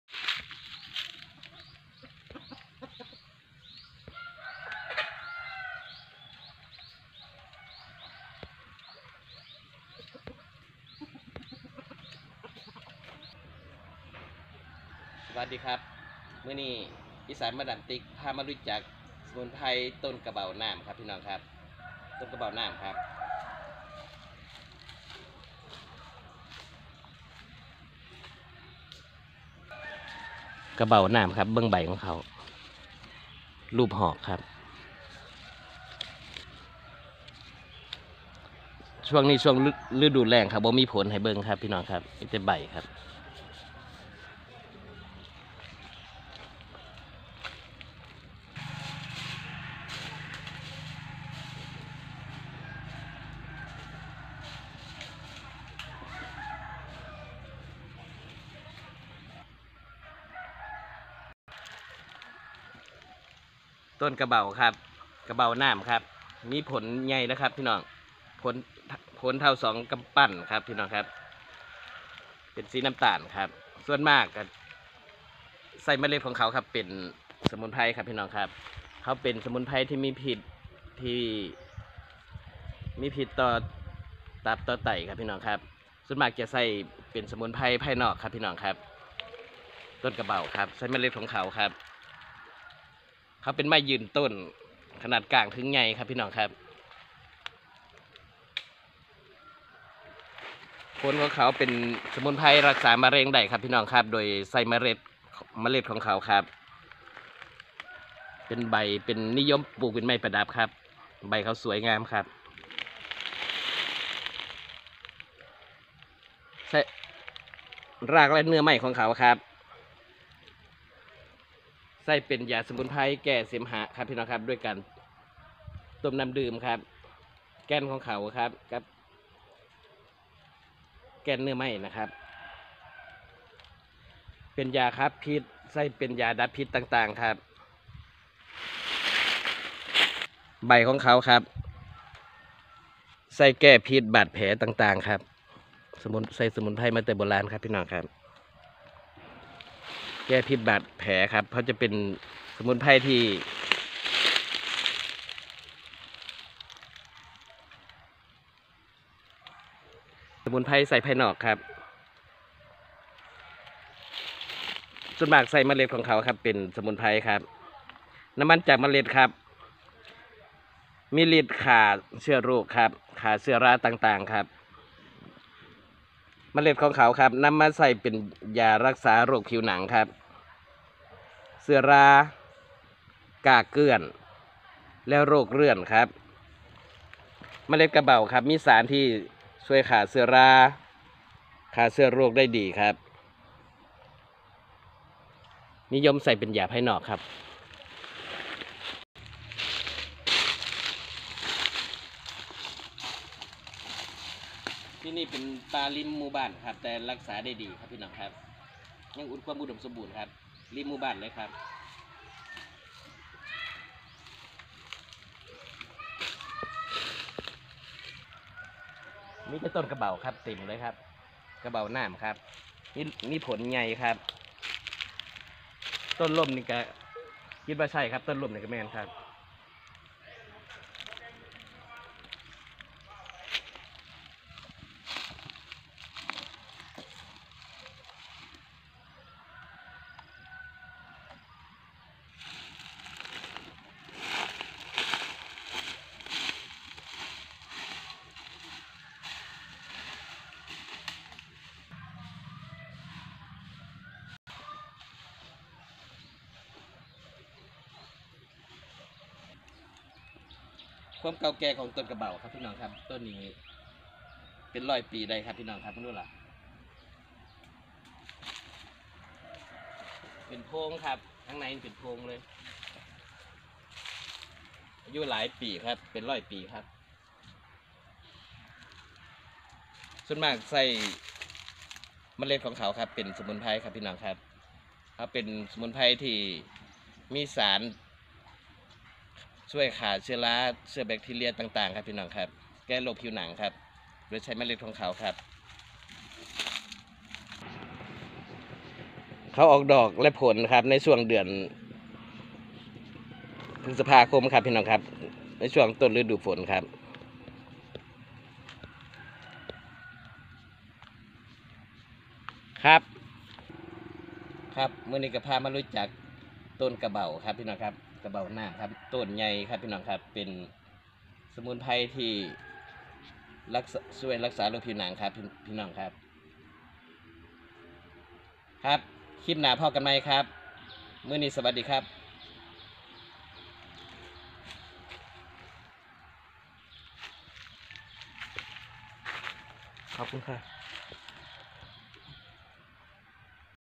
กระเบาน้ำ สมุนไพรให้สารยับยั้งเชื้อราเชื้อโรค ช่วยโรคผิวหนัง
กระเบาน้ำ เป็นไม้ยืนต้นที่ให้ร่มเงาให้ดี กระเบามีผลขนาดใหญ่เปลือกผลสีน้ำตาล น้ำมันจากเมล็ดกระเบามีฤทธิ์ช่วยกำจัดเชื้อโรคเชื้อราต่างๆสรรพคุณช่วยรักษาโรคผิวหนังใด้ดี ใช้รักษาโรคมะเร็ง ใบและแก่นมีสรรพคุณรักษาบาดแผลสด